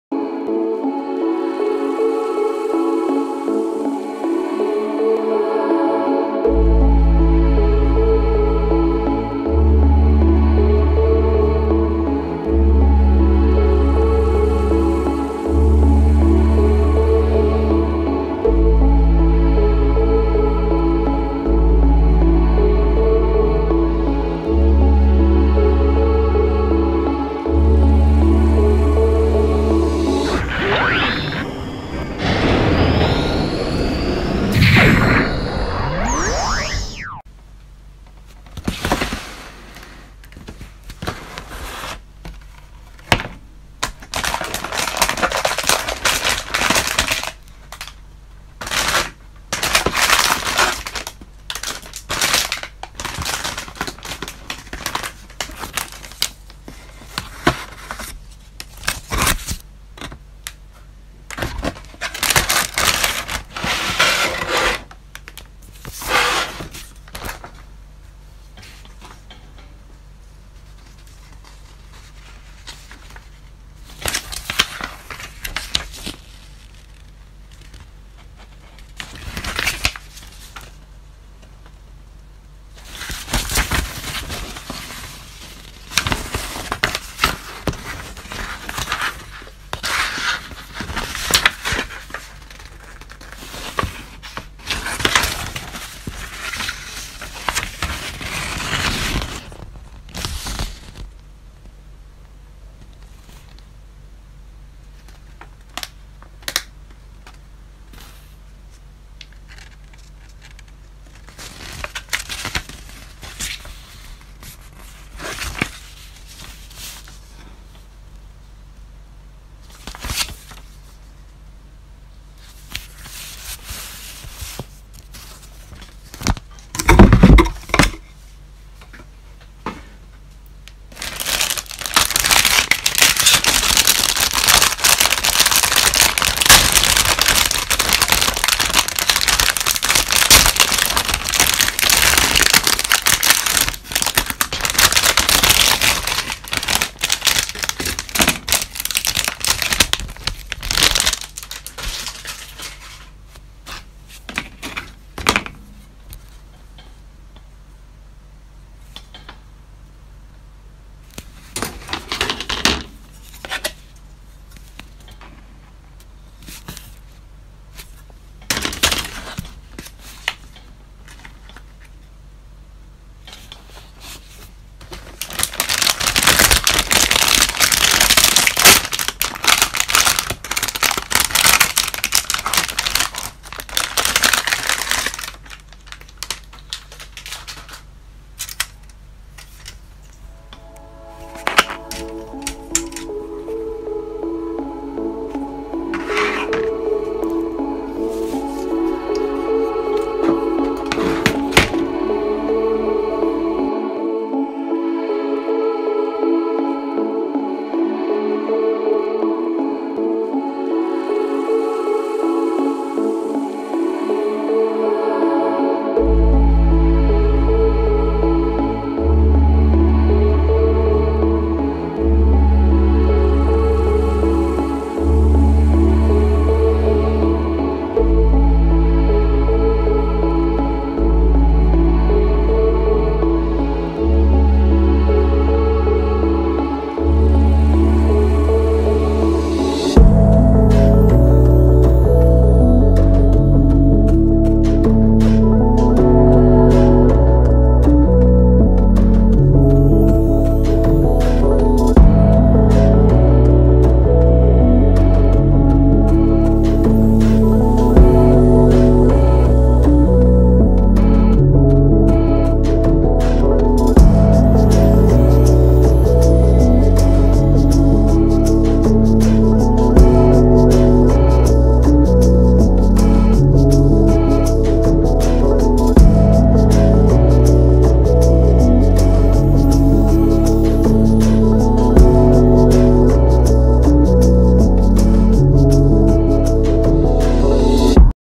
ASMR Unboxing Entry Grade Nu Gundam | RX-93 νガンダム
ASMR Unbox Entry Grade Nu Gundam, one of the most popular Gundam in world and this model kit is beginner friendly!

Details

1) Putaro Plastic Modeller (Gunpla/Toys)